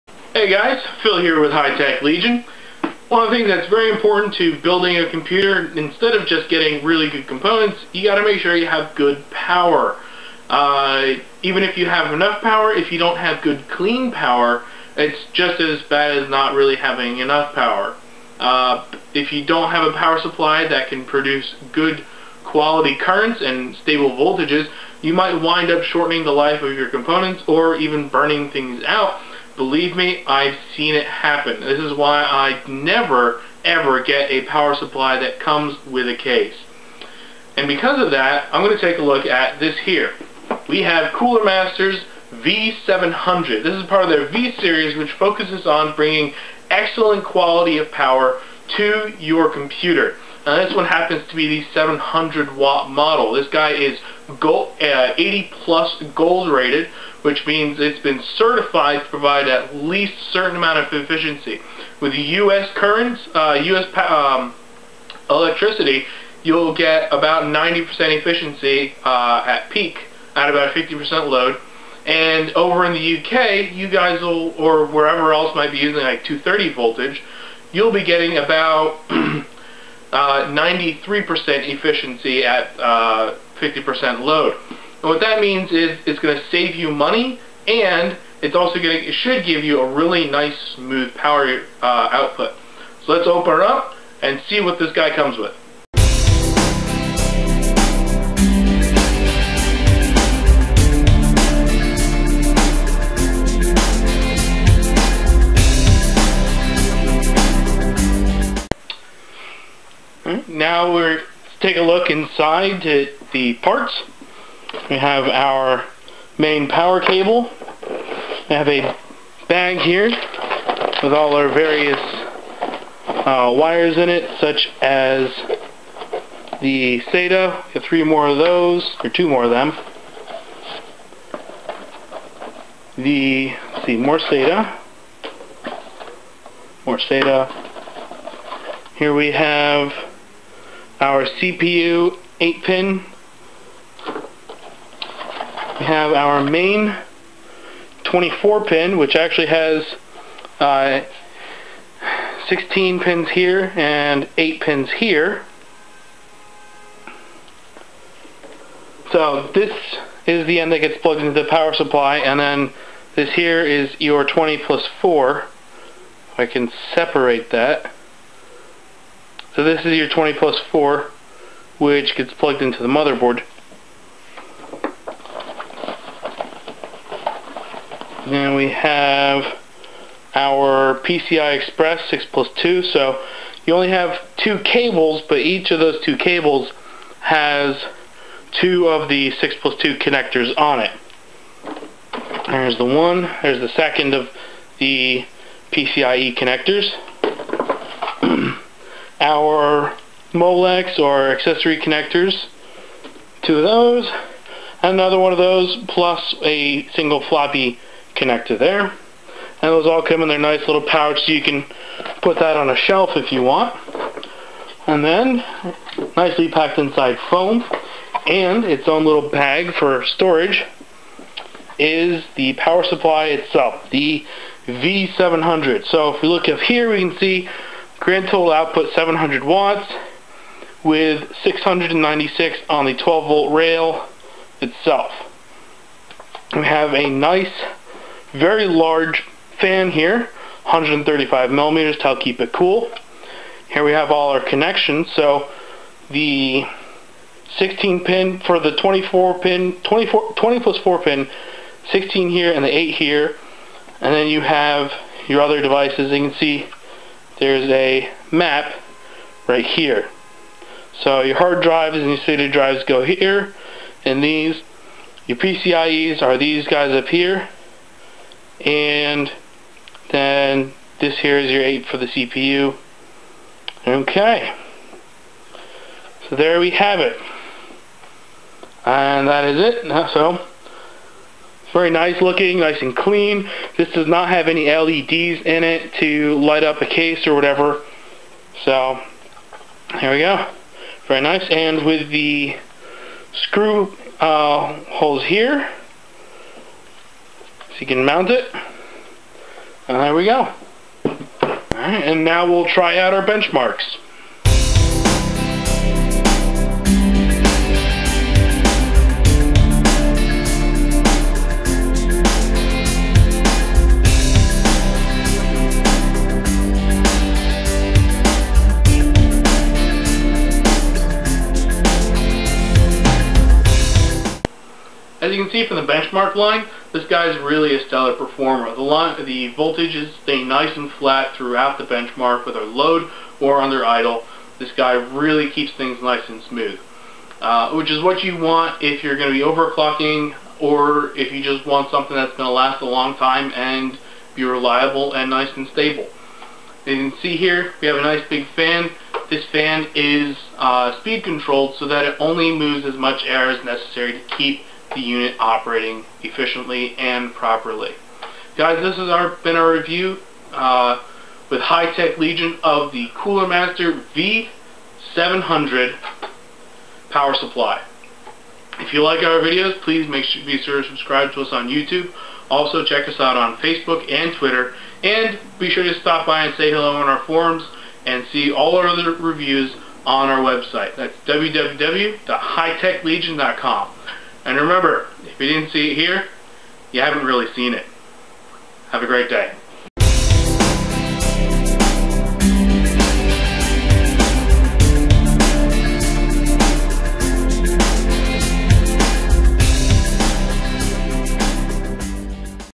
Cooler Master V-Series 700 Watt Power Supply Review, Unboxing and Benchmarks
The Cooler Master V-Series V700 is an 80 PLUS Gold certified power supply. Being 80 PLUS Gold certified means the V700 under a 50% load will operate at 90% efficiency!

There's a big 135mm fan inside to keep it both cool and quiet. This all means that it'll joyfully run a pair of high-end graphics cards with the four 6+2 PCIe connectors, and keep any processor happy with its 4+4 pin 12v connector. Cooler Master also says that the 12v rail will only fluctuate by 1%, a key feature to provide clean power.

2013 HiTechLegion.com